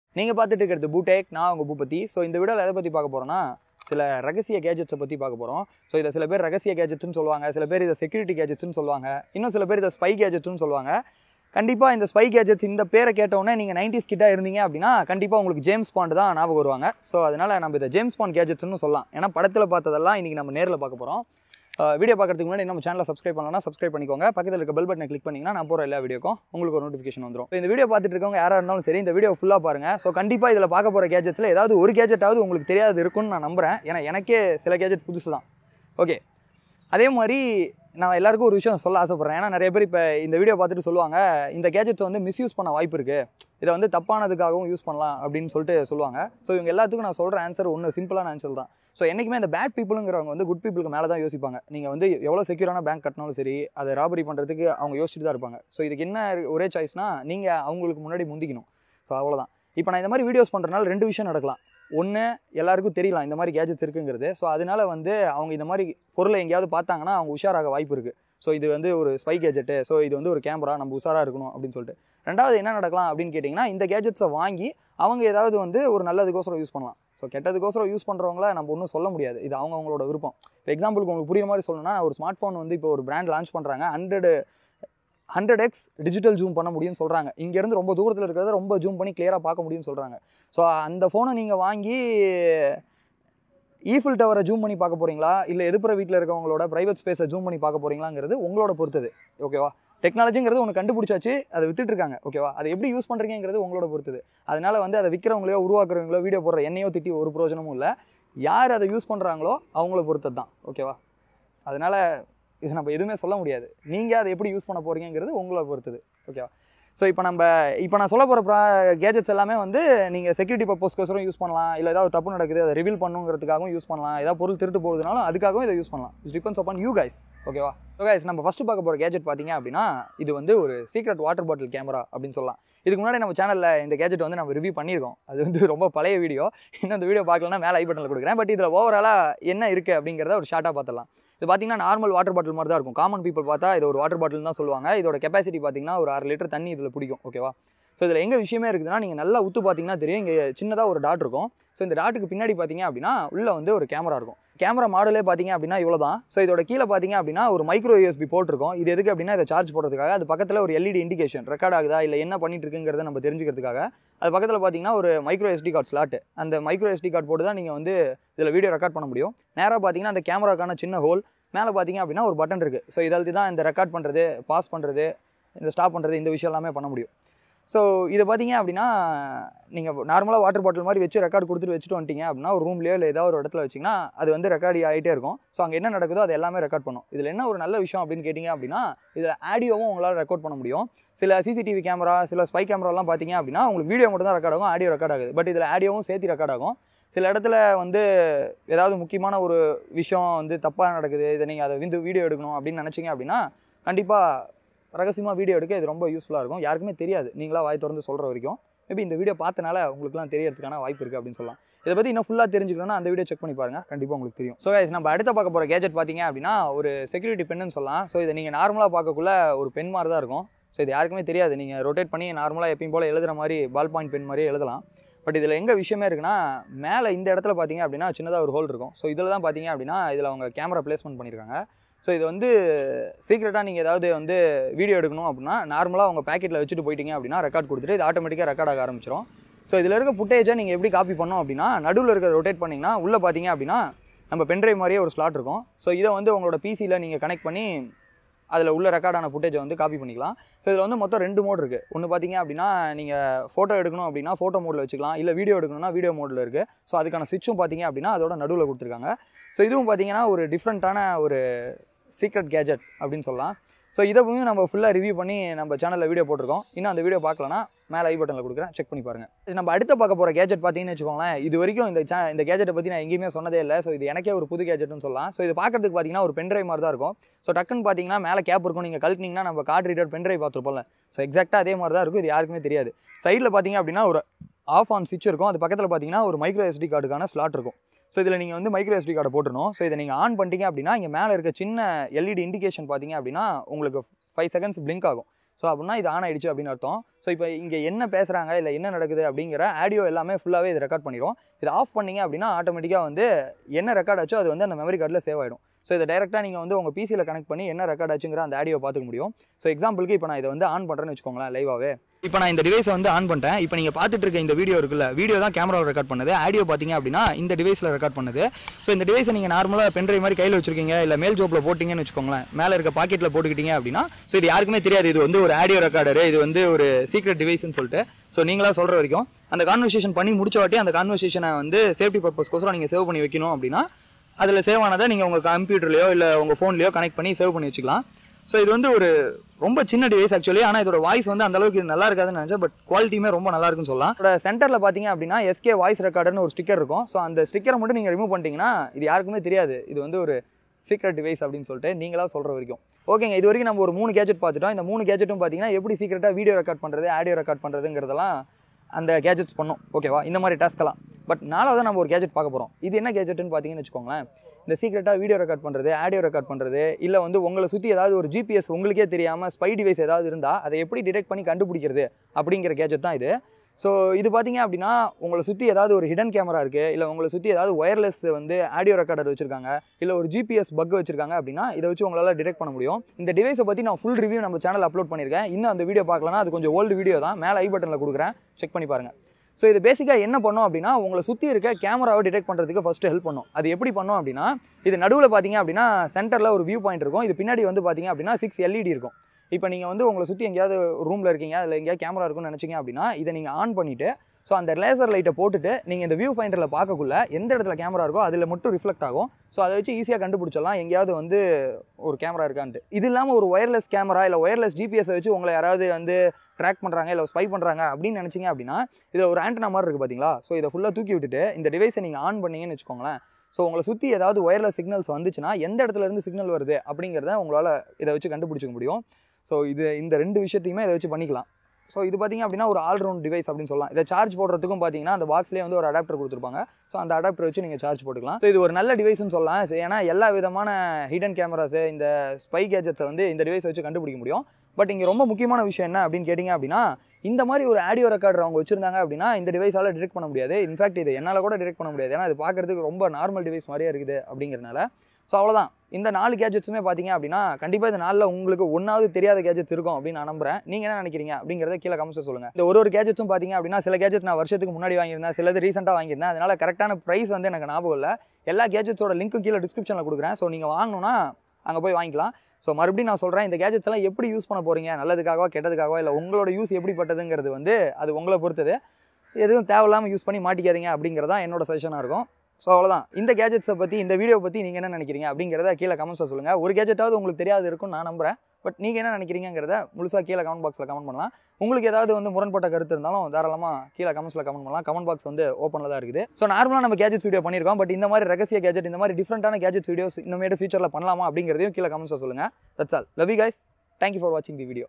4 ரகசிய gadgets நீங்க பார்த்திருக்க மாட்டீங்க !
Audio Voice Recorder

பேனா கேமரா

Camera Detector CC308+

பாட்டில் கேமரா

Gears I use:
My Main Camera
My Canon 24mm Lens
My Canon 50mm Prime Lens
My Main Mic
My Lav Mic
My Main Tripod
My vlogging mic